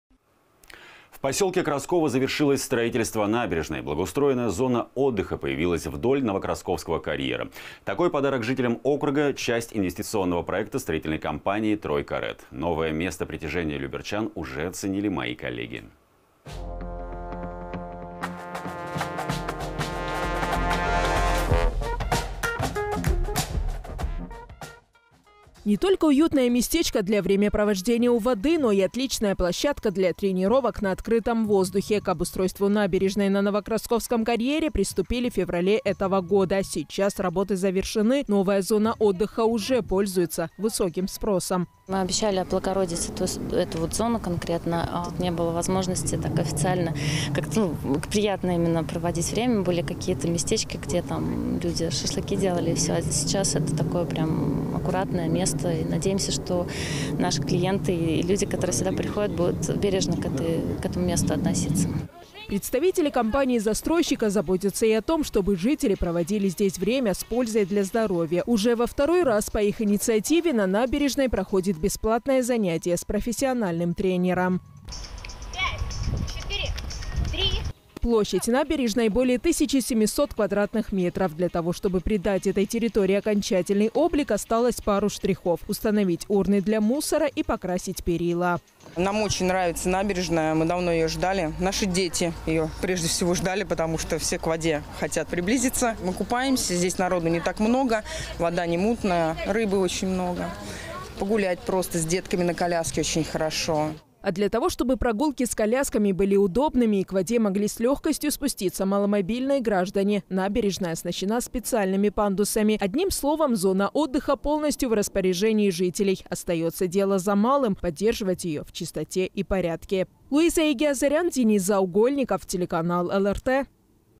Новая точка притяжения
В поселке Красково завершилось строительство набережной. Благоустроенная зона отдыха появилась вдоль Новокрасковского карьера. Такой подарок жителям округа – часть инвестиционного проекта строительной компании «3-RED». Новое место притяжения люберчан уже оценили мои коллеги.

Не только уютное местечко для времяпровождения у воды, но и отличная площадка для тренировок на открытом воздухе. К обустройству набережной на Новокрасковском карьере приступили в феврале этого года. Сейчас работы завершены. Новая зона отдыха уже пользуется высоким спросом.

Мария Хурамшина, директор по маркетингу компании «3-RED»:
«Мы обещали облагородить эту зону конкретно. Тут не было возможности вот так, официально, приятно проводить время. Были какие-то местечки, где люди шашлыки делали . А сейчас это такое аккуратное место, надеемся, что наши клиенты и люди, которые сюда приходят, будут бережно к этому месту относиться».

Представители компании-застройщика заботятся и о том, чтобы жители проводили здесь время с пользой для здоровья. Уже во второй раз по их инициативе на набережной проходит бесплатное занятие с профессиональным тренером.

Площадь набережной – более тысячи семисот квадратных метров. Для того, чтобы придать этой территории окончательный облик, осталось пару штрихов – установить урны для мусора и покрасить перила.

Светлана Сняткова, жительница ЖК «Новокрасково» г.о. Люберцы:
«Нам очень нравится набережная, мы давно ее ждали. Наши дети ее, прежде всего, ждали, потому что все к воде хотят приблизиться. Мы купаемся, здесь народу не так много, вода не мутная, рыбы очень много, погулять просто с детками на коляске очень хорошо».

А для того, чтобы прогулки с колясками были удобными и к воде могли с легкостью спуститься маломобильные граждане, набережная оснащена специальными пандусами. Одним словом, зона отдыха – полностью в распоряжении жителей. Остается дело за малым – поддерживать ее в чистоте и порядке.